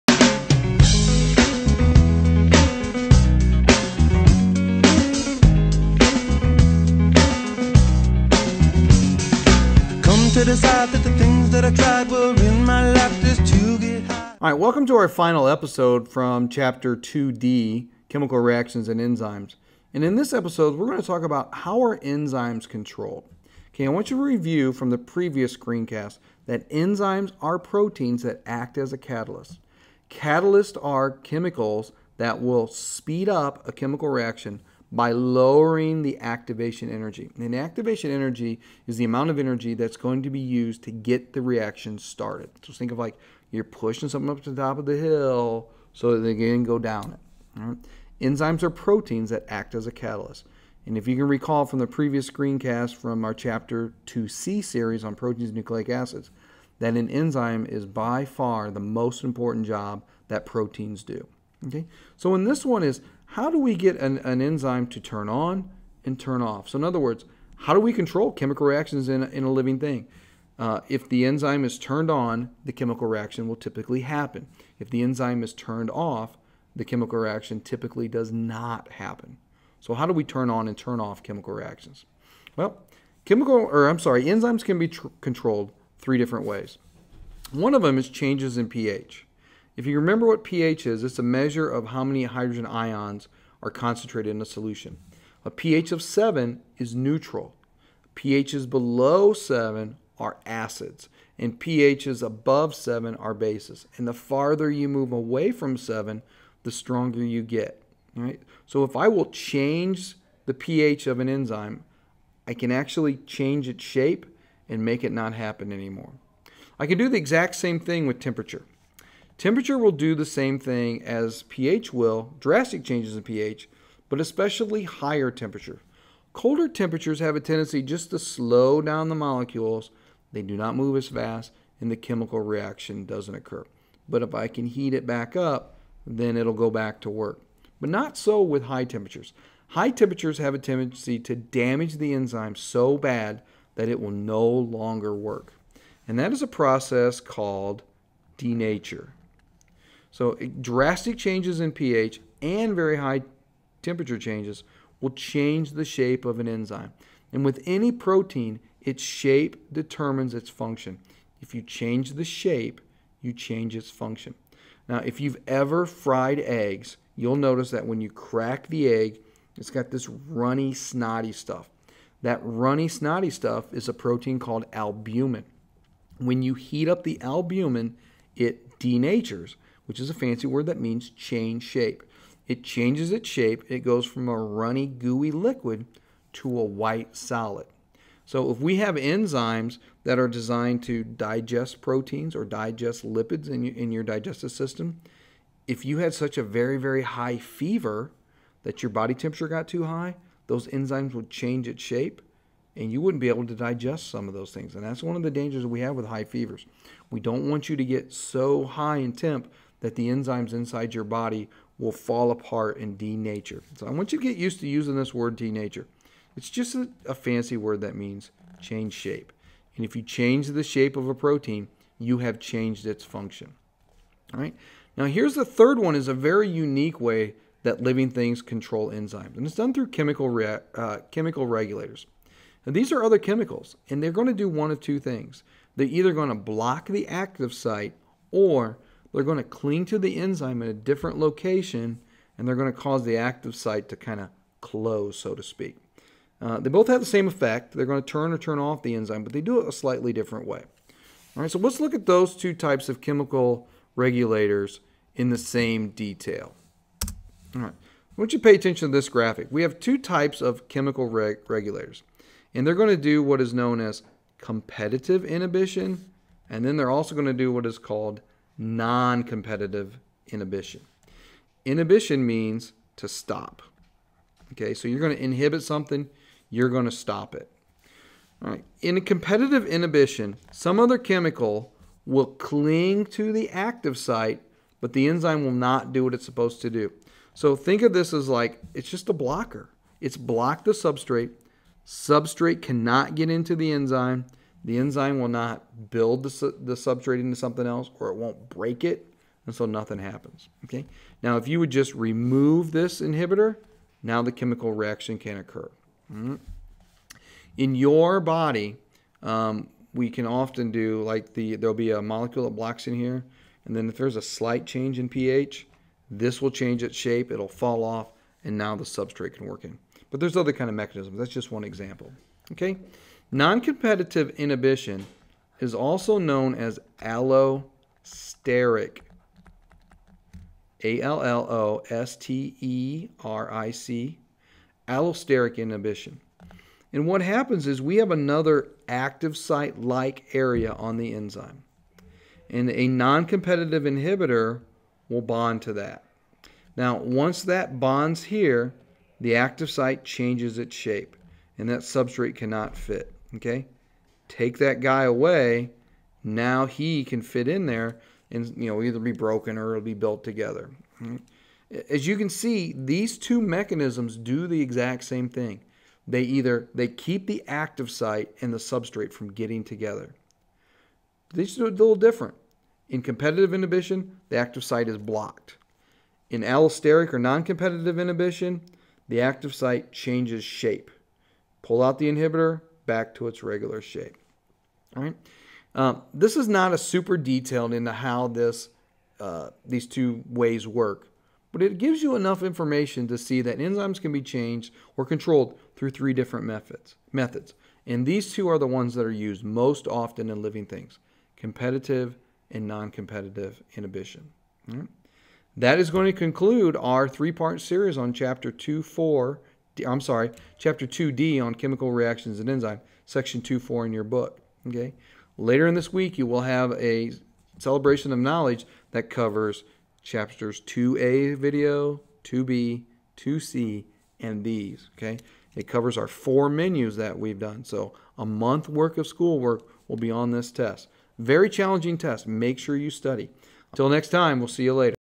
Chapter 2D Part 3 - Controlling Enzymes
The final episode in this series on enzymes will explain how enzymes can be controlled by changes in pH and temperature, but special emphasis will be placed on how enzymes are controlled by using chemical regulators is competitive and allosteric inhibition.